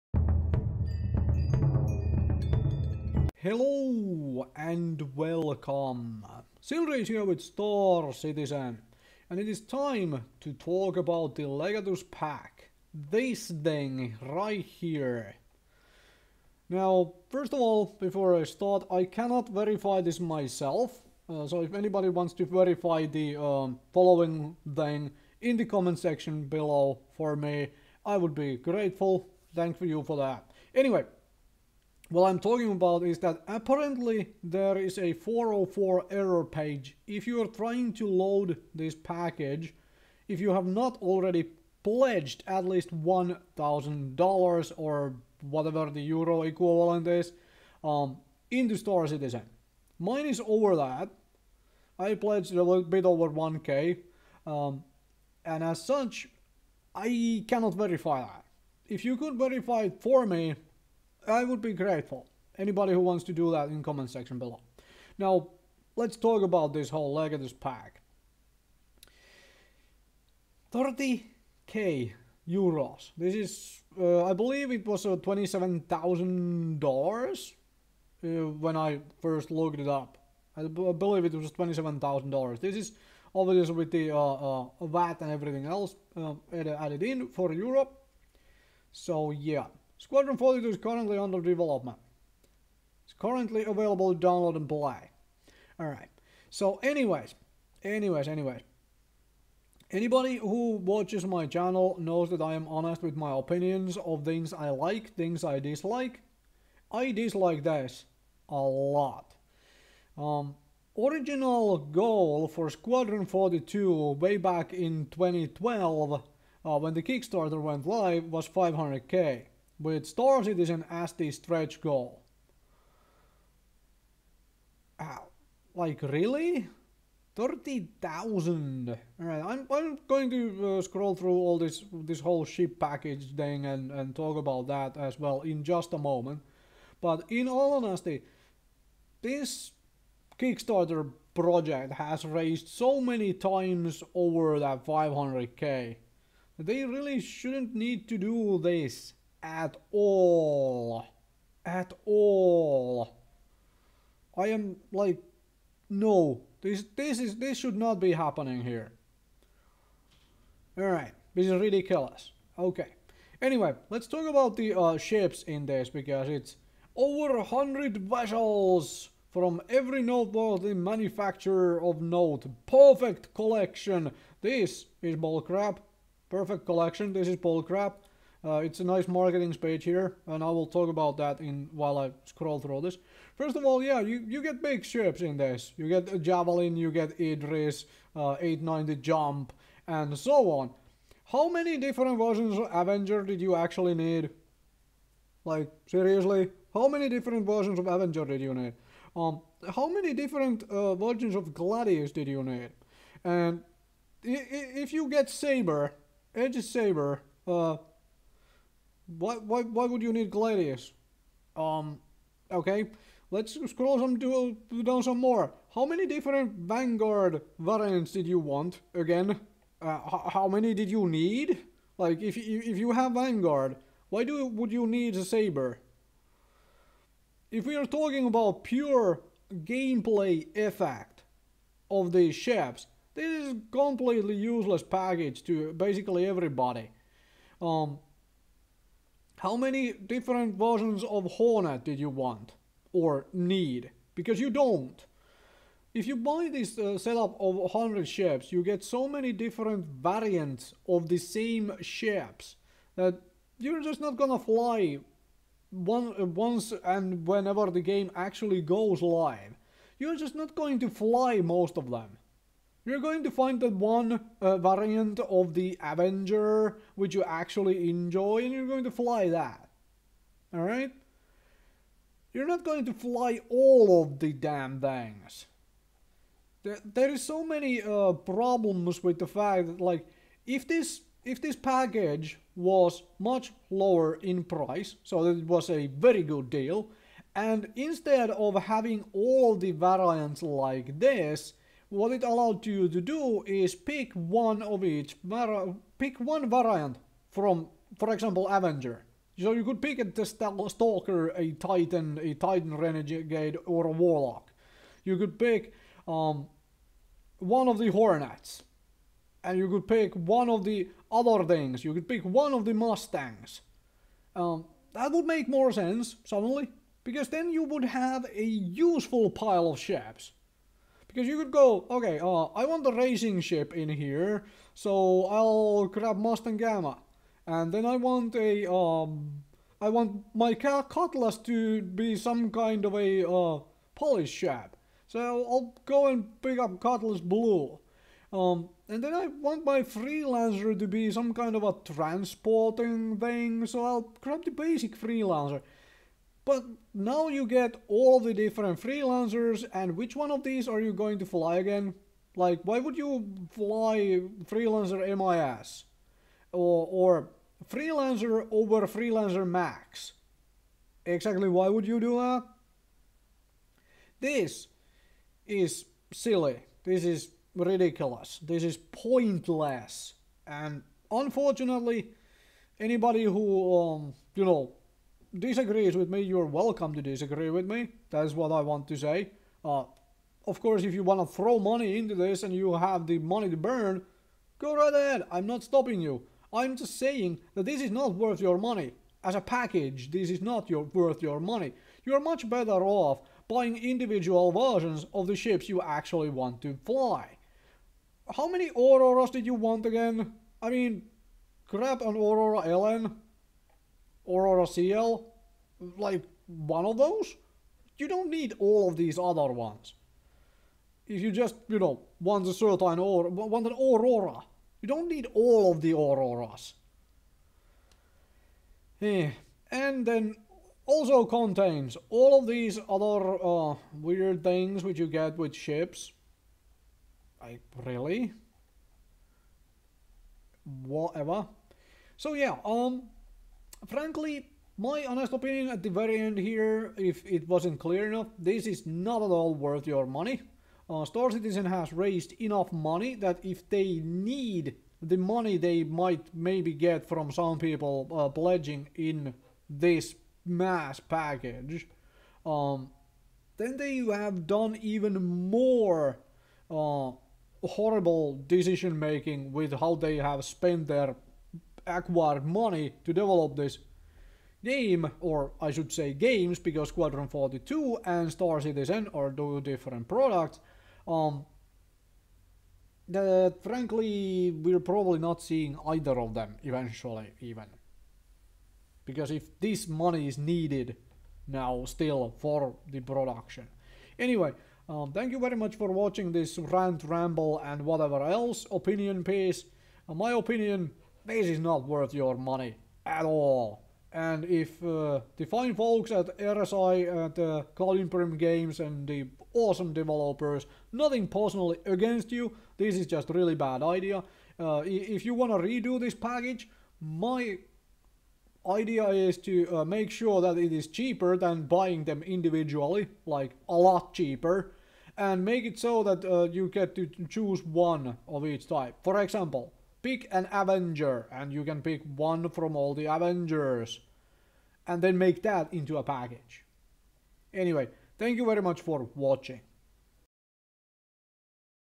Star Citizen - Legatus Pack Talk
My thoughts on the 30k ship pack.

for 5000 free UEC upon registering!